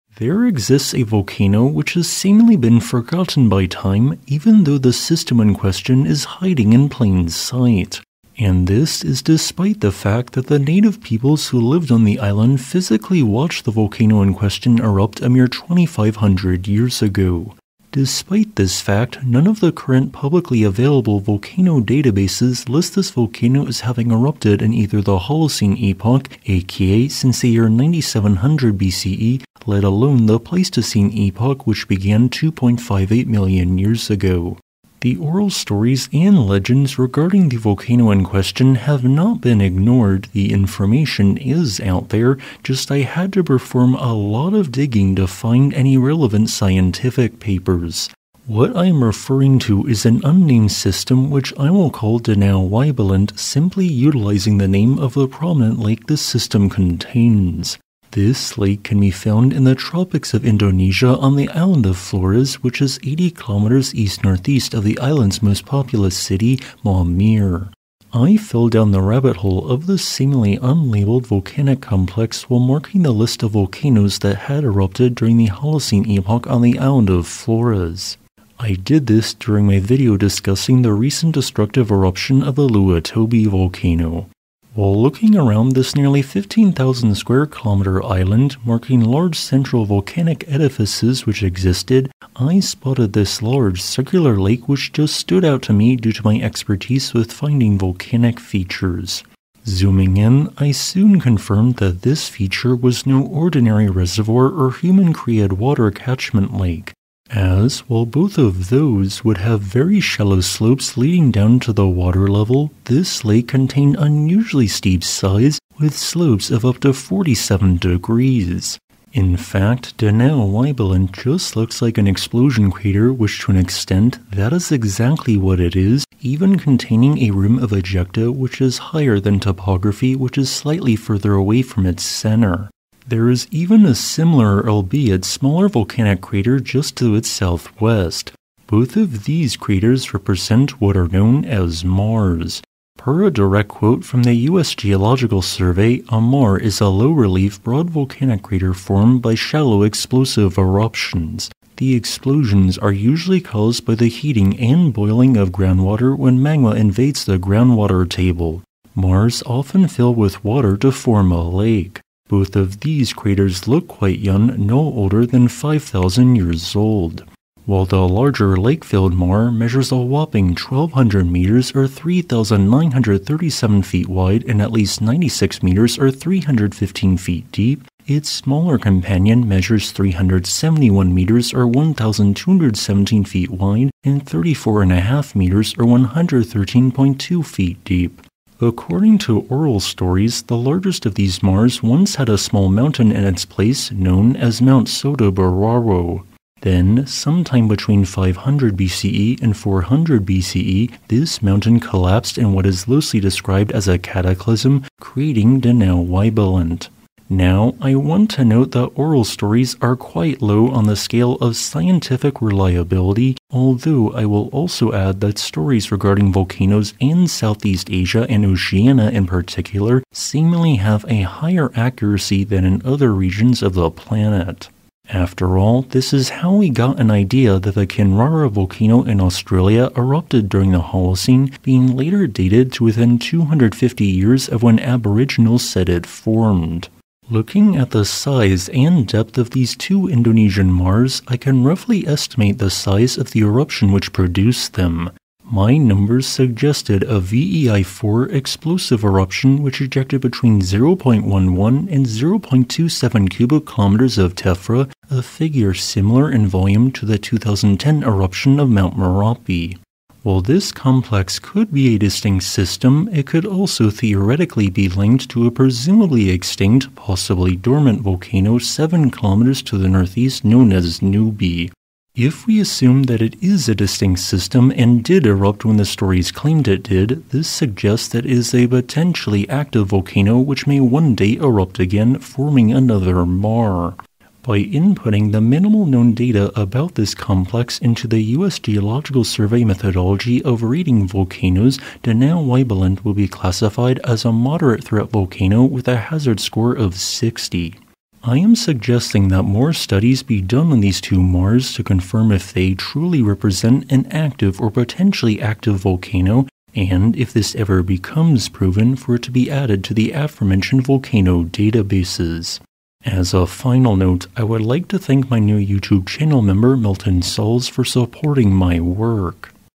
The Volcano Forgotten by Time; Hiding in Plain Sight
There exists a volcano which despite being witnessed erupting by locals during the early metal age is today not listed in any volcano databases. In other words, despite oral tales about its existence, it has seemingly been forgotten by science in the more than 2,000 years since. This video hopes to highlight the volcano in question, which I am calling Danau Waibelen. This complex of maars may in fact not have erupted when the oral stories suggested, but due to its young appearance warrants further scientific studies. This volcano is located on Indonesia's island of Flores.

Sources/Citations:
[1] J. Ewert, A. Diefenbach, D. Ramsey, "2018 Update to the U.S. Geological Survey National Volcanic Threat Assessment", U.S. Geological Survey, Accessed October 22, 2022.

0:00 A Missing Volcano
0:58 Danau Waibelen
2:12 Maars
3:57 Eruption Size
4:44 Hazard Rating